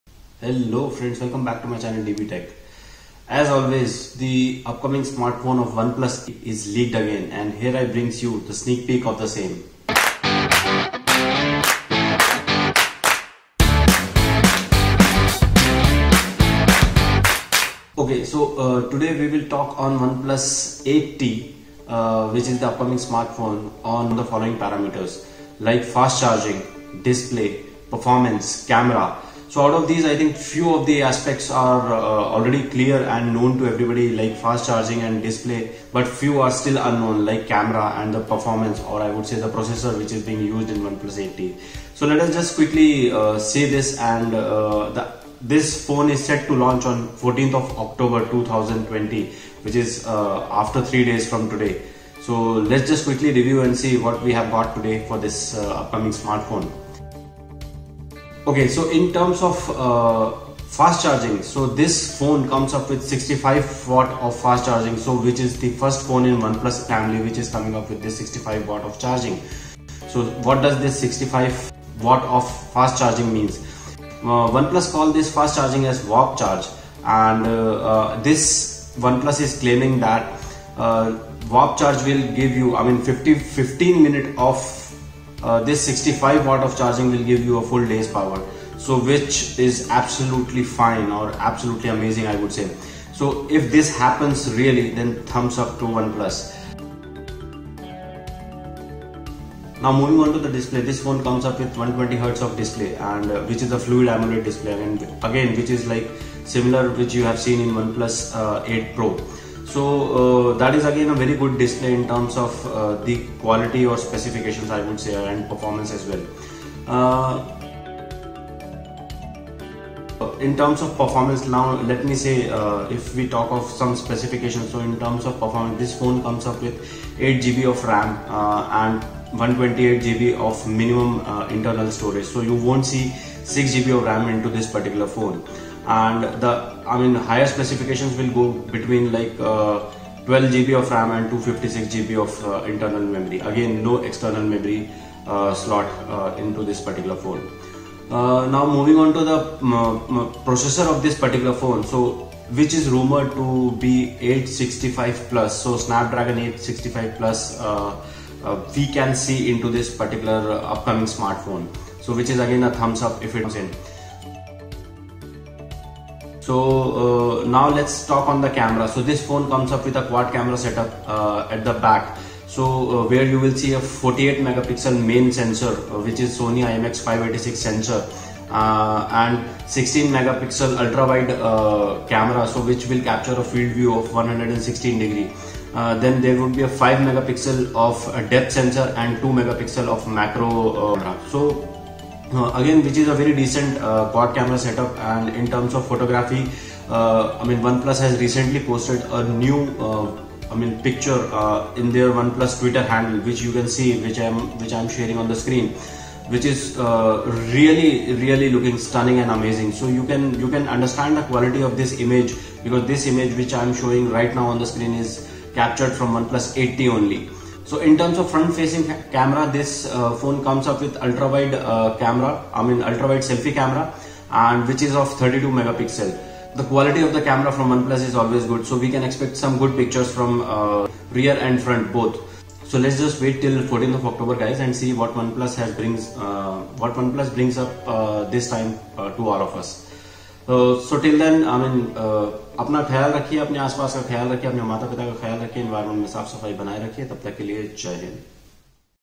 One Plus 8T Leaked? Here's Sneak Peak.
One plus upcoming smart phone is leaked again & here i bring you the sneak peak basis all the rumours.
This flagship phone comes with multiple new things which was not available in oneplus phones so far. This phone comes with 65W of fast charging which is 1st in One plus family.

DISPLAY

Type                Fluid AMOLED, 120Hz, HDR10+

PLATFORM

OS         Android 11, HydrogenOS 11
CPU       Octa-core (1x2.84 GHz Kryo 585 & 3x2.42 GHz Kryo 585 & 4x1.8
               GHz Kryo 585)

MAIN CAMERA

Quad       48 MP, f/1.8, 1/1.43", 1.12µm, omnidirectional PDAF, Laser AF, OIS
                16 MP, f/2.2, 123˚ (ultrawide)
                 5 MP, f/2.4, (macro)
                 2 MP, f/2.4, (depth)
Features  Dual-LED flash, HDR, panorama
Video 4K@30/60fps, 1080p@30/60/240fps, Auto HDR, gyro-EIS

SELFIE CAMERA

Single      32 MP
Features Auto-HDR
Video      1080p@30fps, gyro-EIS

Source
1) Trakin Tech :-This Is The OnePlus 8T 5G | Guess The Price 💸 5X Giveaway ⚡⚡⚡ 6M Special

2) TT Technology :- ONEPLUS 8T - This Is It!